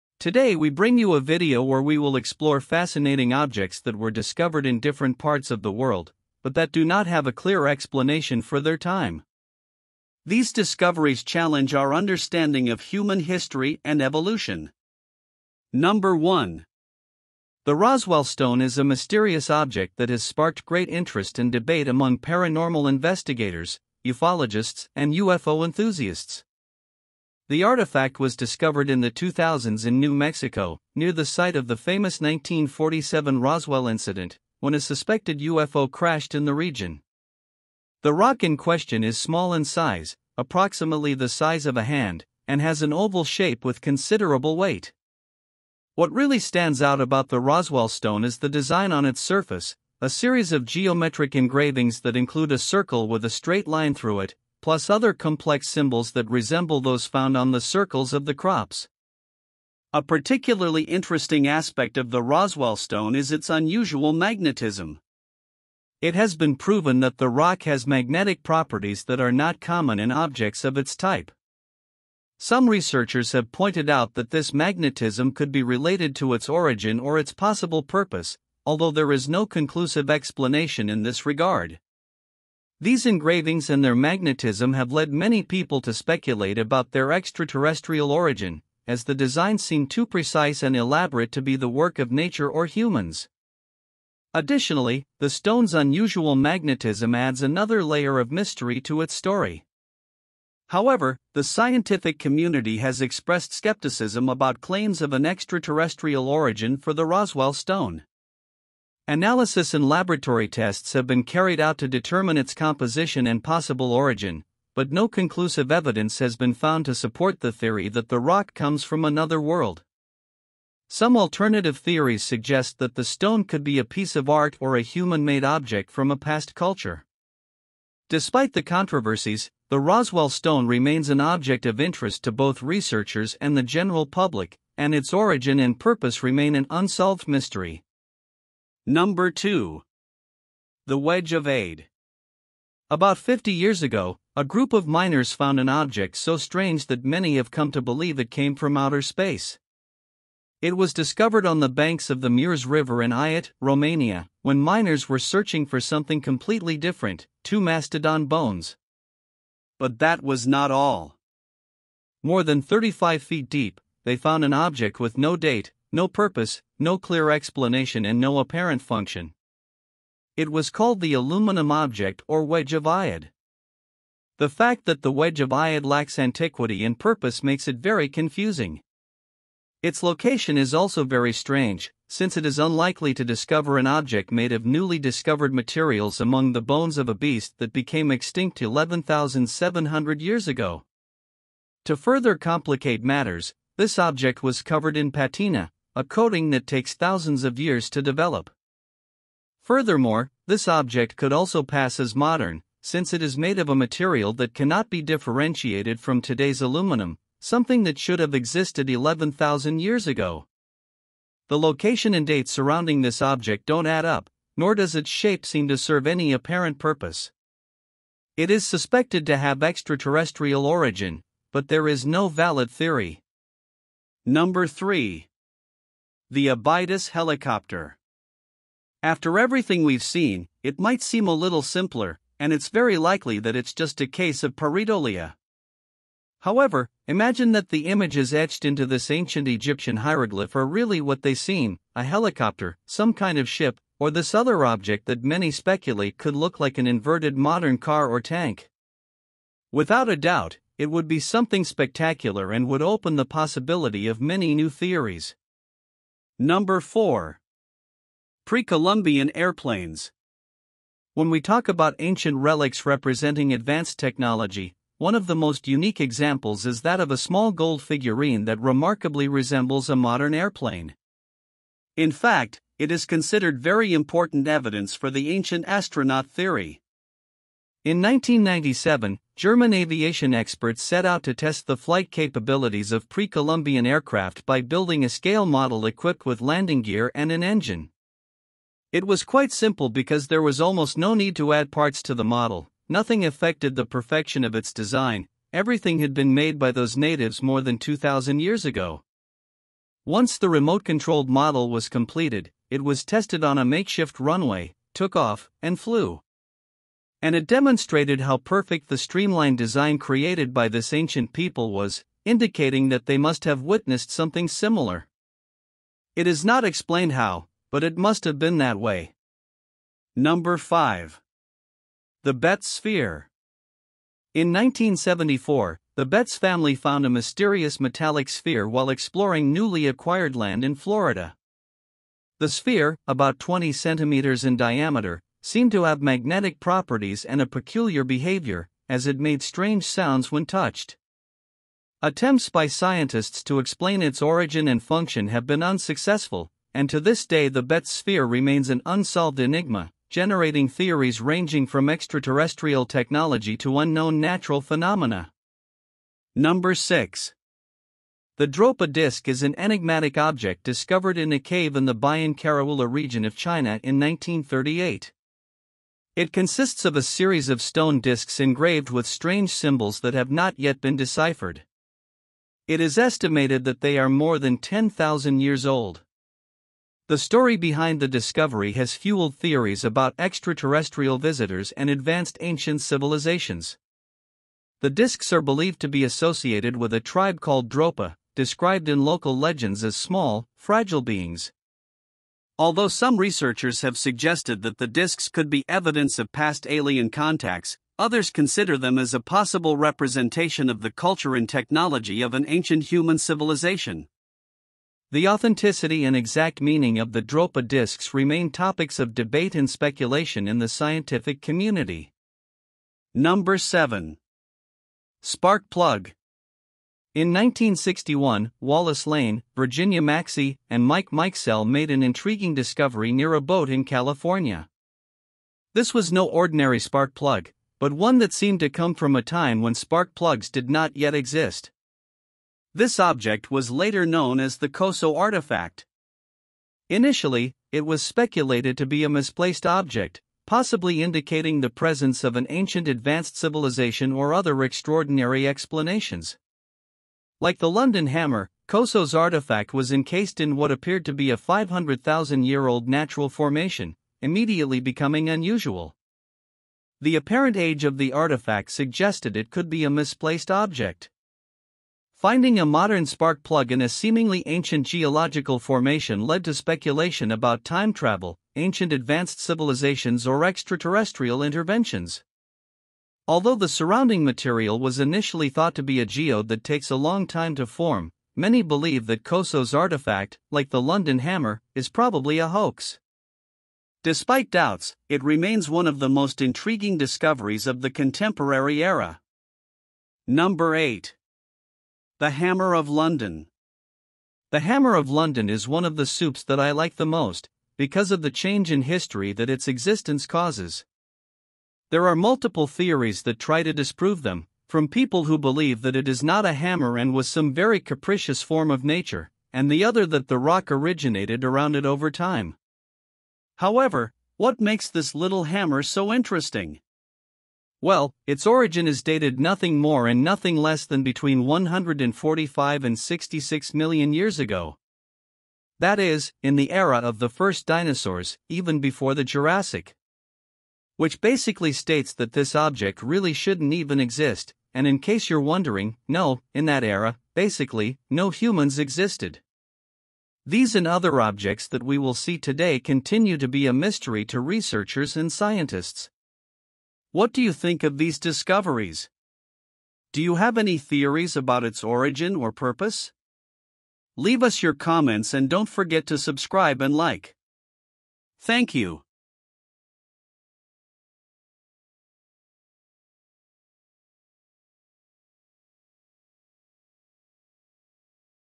Strange archaeological discoveries that terrify scientists
Today we bring you a video where we will explore fascinating objects that were discovered in different parts of the world, but that do not have a clear explanation for their time. These discoveries challenge our understanding of human history and evolution.